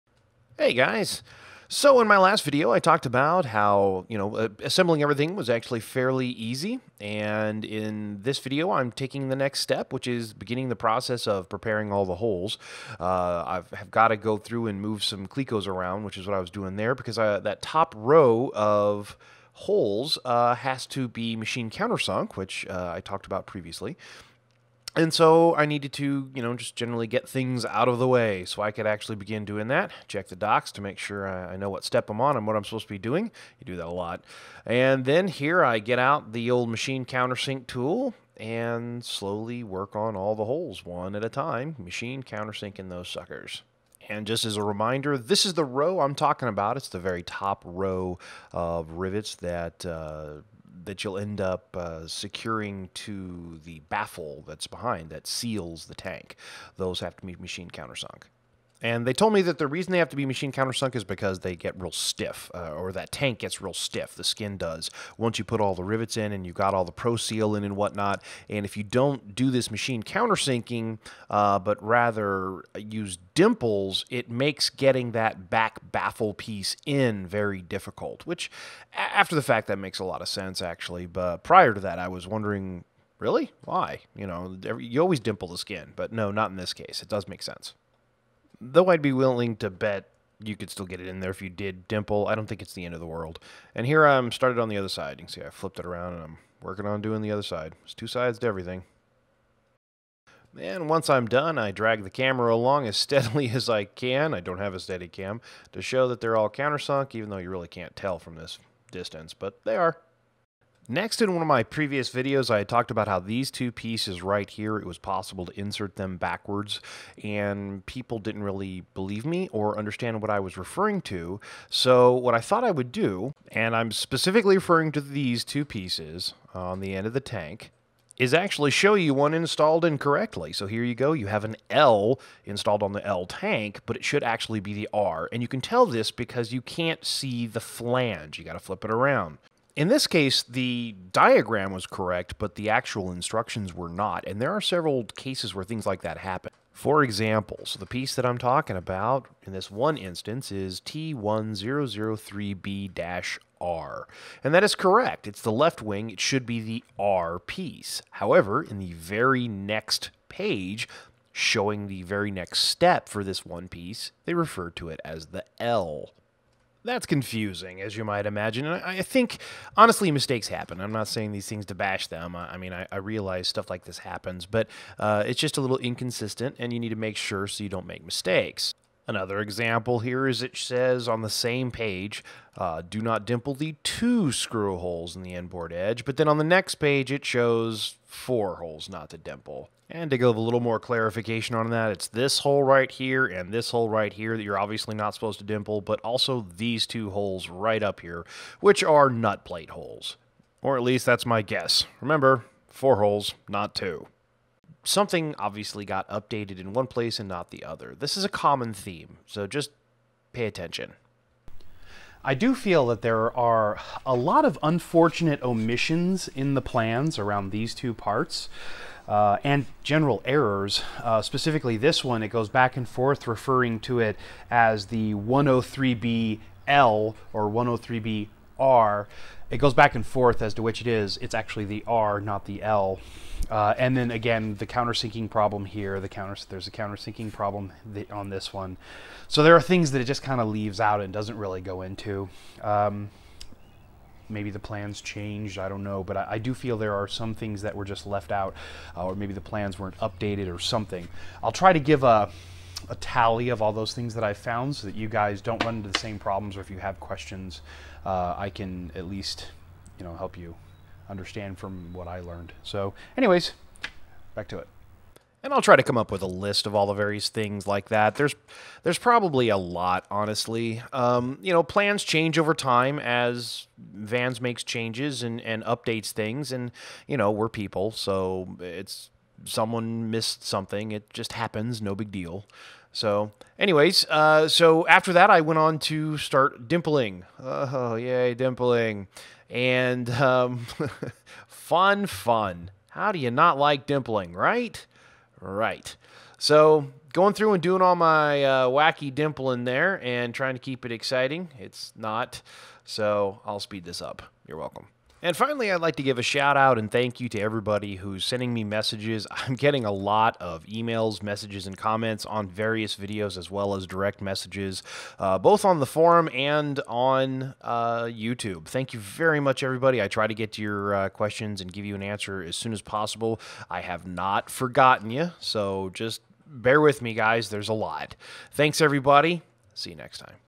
RV-10 Wings - 023 - Left wing tank counter sinking and dimpling
In this installment I go through and work on the fuel tank.  Specifically machine countersinking and dimpling the holes!  So many holes!

I also point out a couple minor errors in the plans that you need to be cognizant of as well as a couple pieces you can install backwards if you're not careful.

I talk about a few unfortunate omissions on a couple of the parts in the plans.  But fear not you can totally figure it out.  ( I mean I did.. )

It could also be a very clever plan by them to slowly ween us off extremely detailed plans and let us figure things out a tad on our own.  I dunno.

Either way, just to be clear, I don't want to make it seem like I'm bashing Van's.. I'm not.  I'm a huge fan of Van's.  What they are doing is incredible.   So my pointing this stuff out is more of a way that I can pass on my findings to you so that you don't have any struggles like I did.